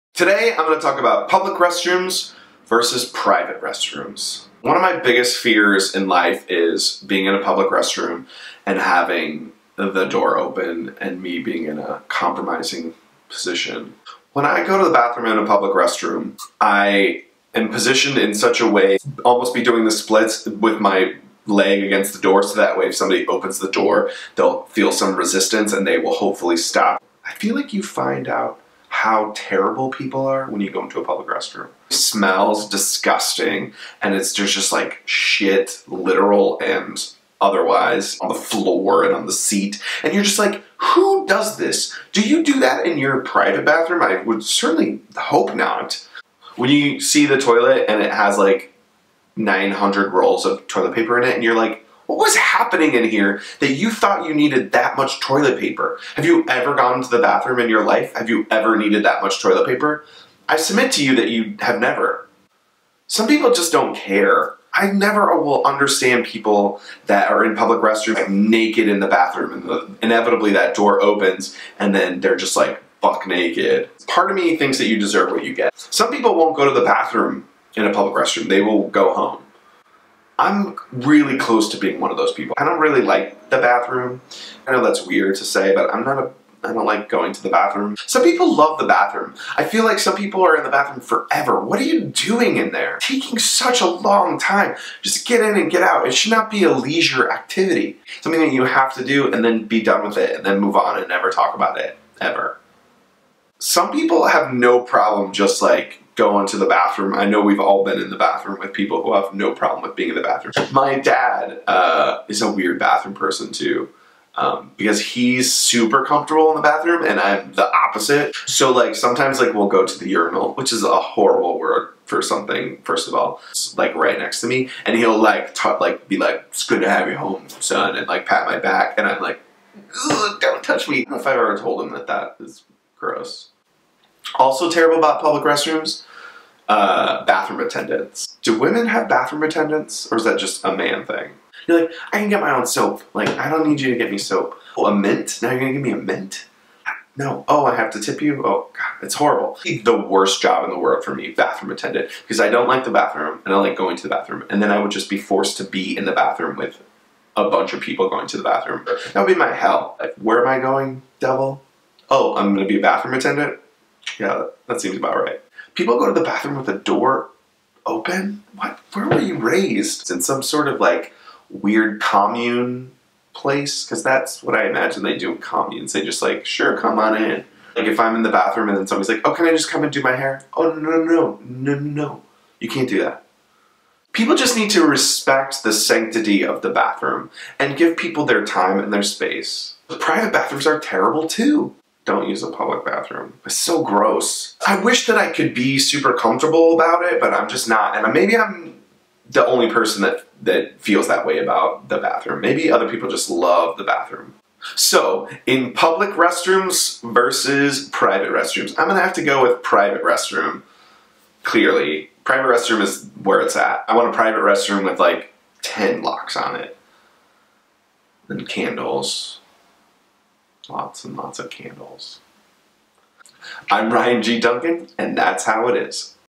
Public VS Private Bathrooms- That's How It Is with Ryan G. Dunkin
Which do you prefer? Can you go to the bathroom anywhere??